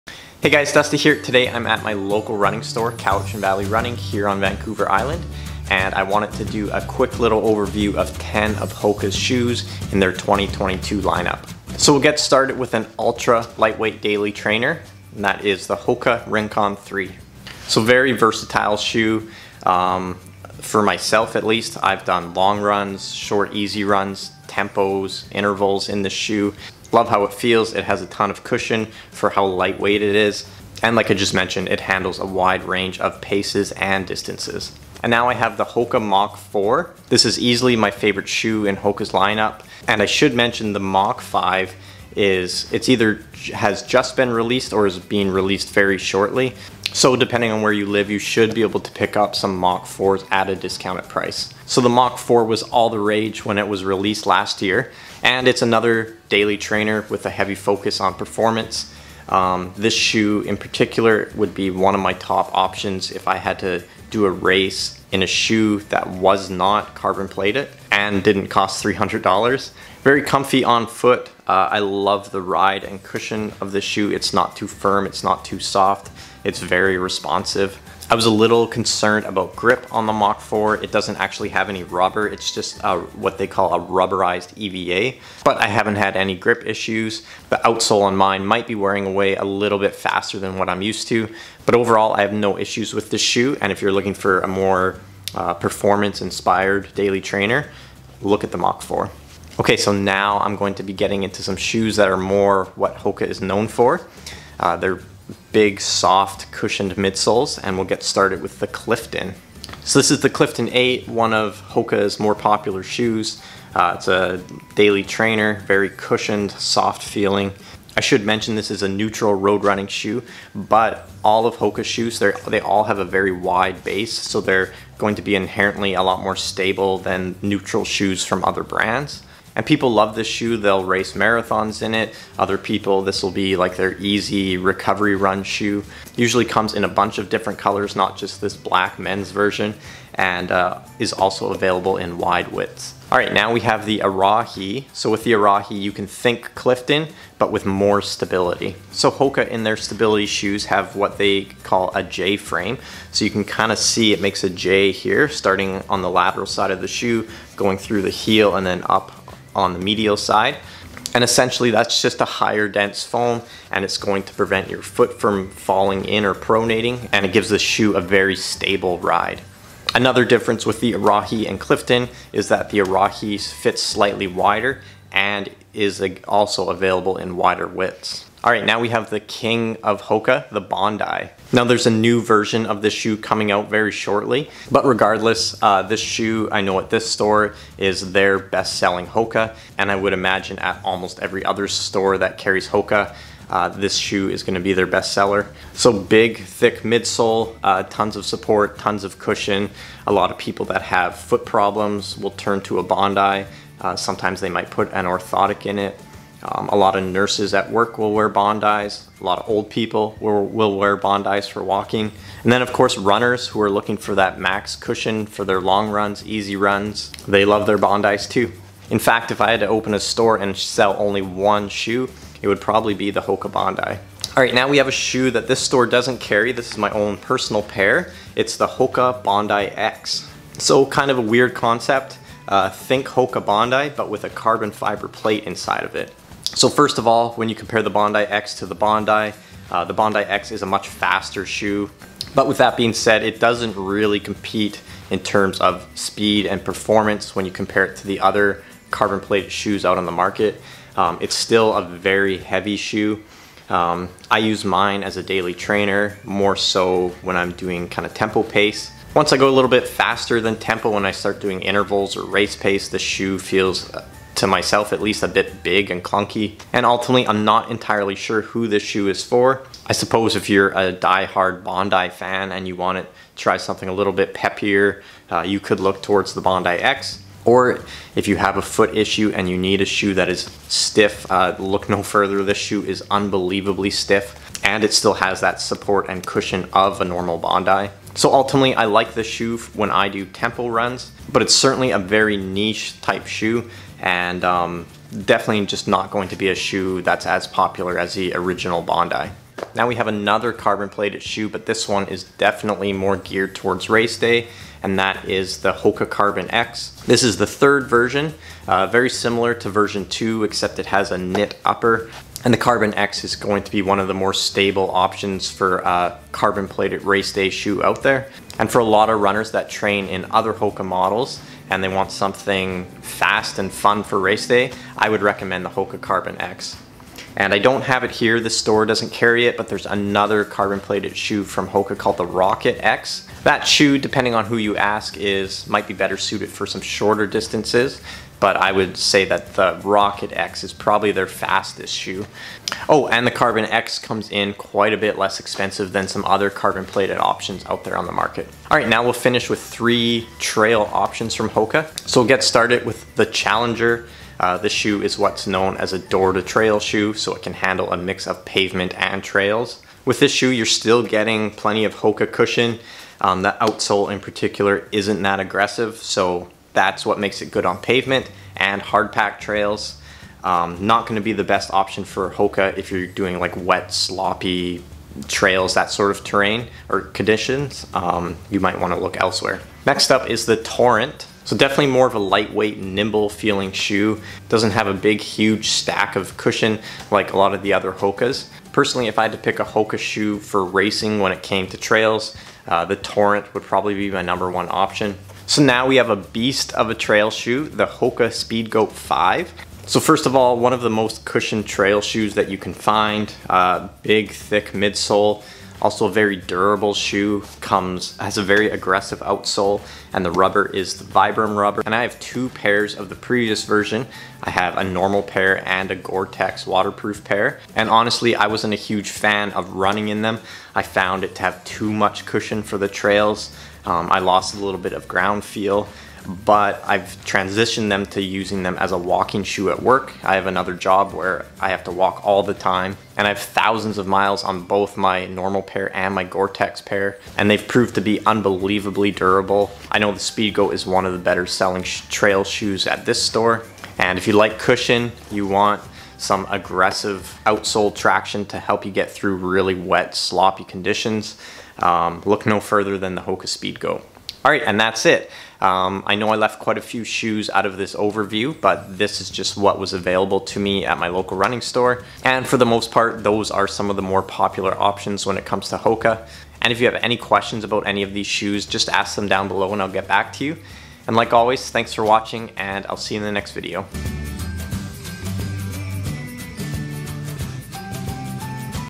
HOKA Running Shoes Review *2022 version*
In this video I give a quick overview of HOKA and most of the models offered. A big thank you to Laura from Cowichan Valley Running for letting me film in her shop. If you live in Canada and want to get a pair of HOKA shoes for yourself, check out the shop here (and save 10% on HOKA shoes by using HOKAREVIEW at checkout):